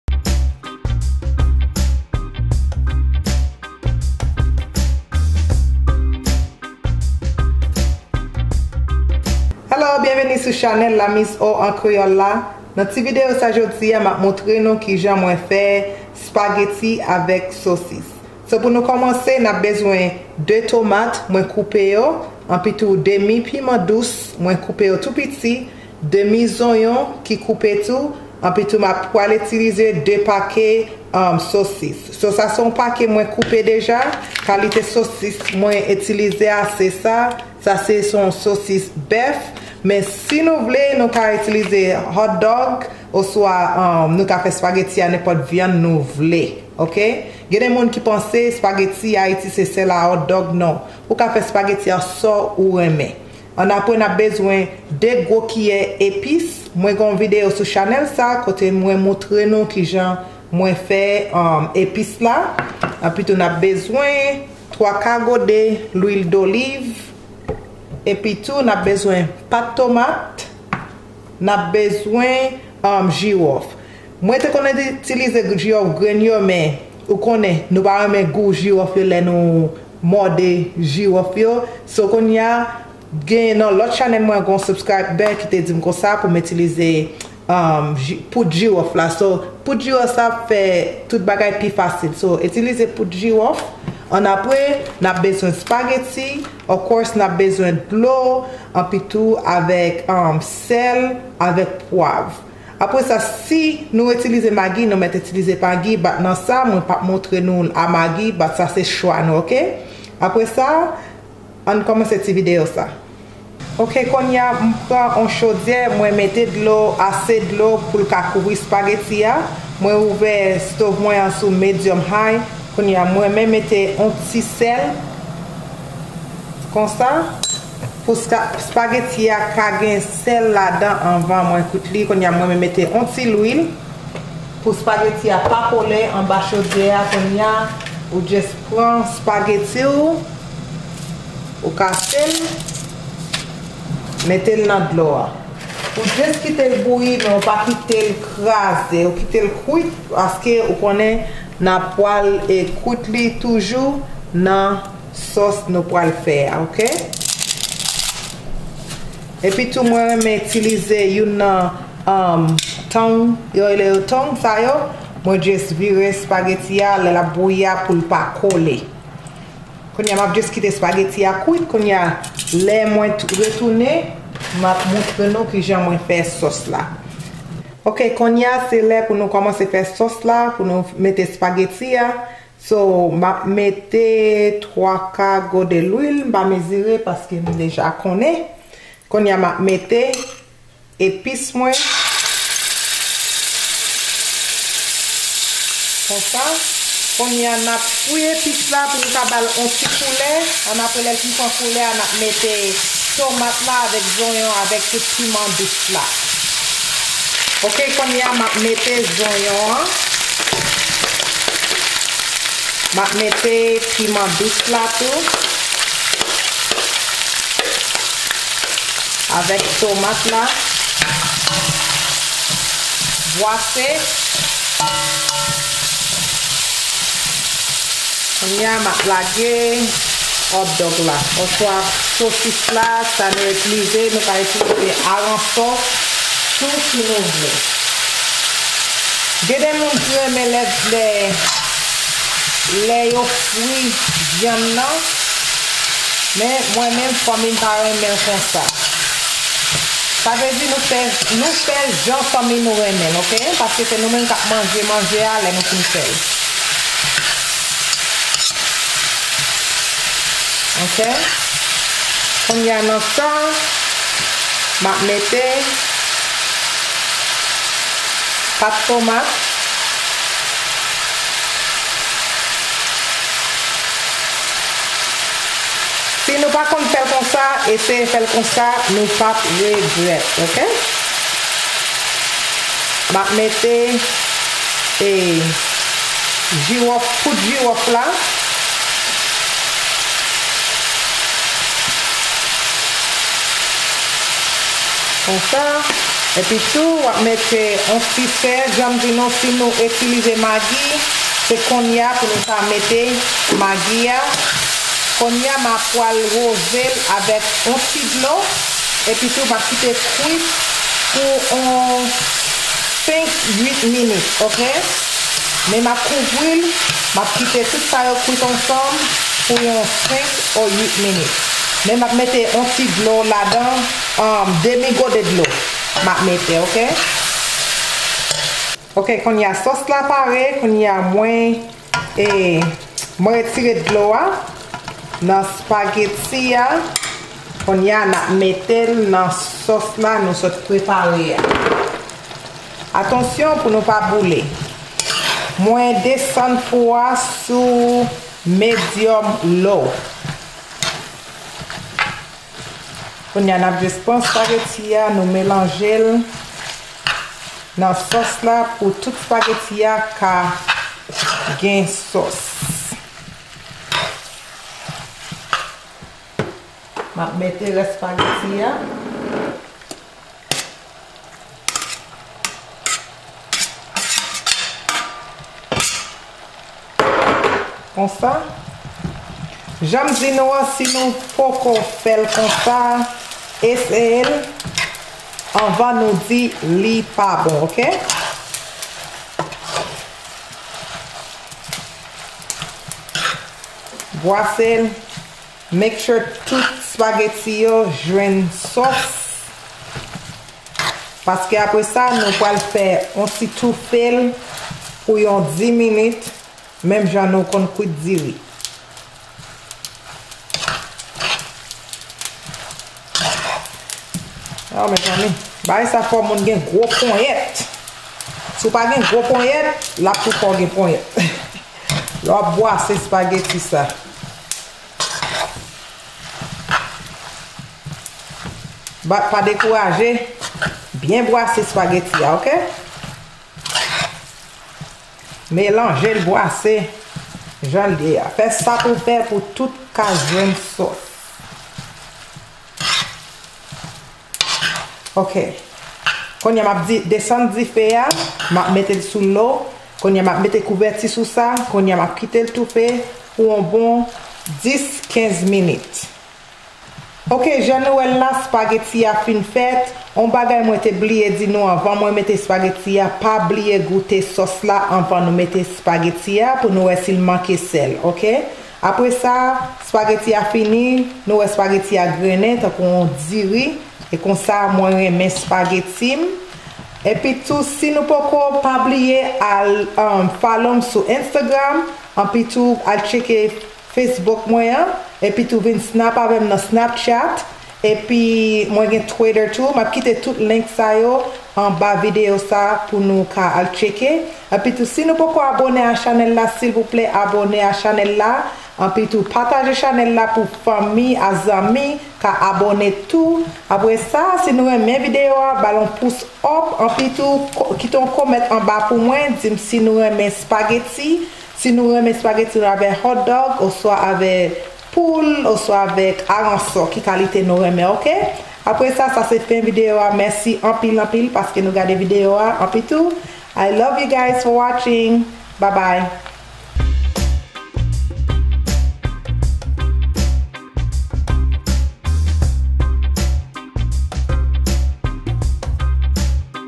Recette Spageti Ak Sosis | Recette Facile | Episode 9 { Creole }
Recette Spageti Ak Sosis | Recette Facile [ Creole] Episode 9
M'ap moutre nou recette mwen remen an creole.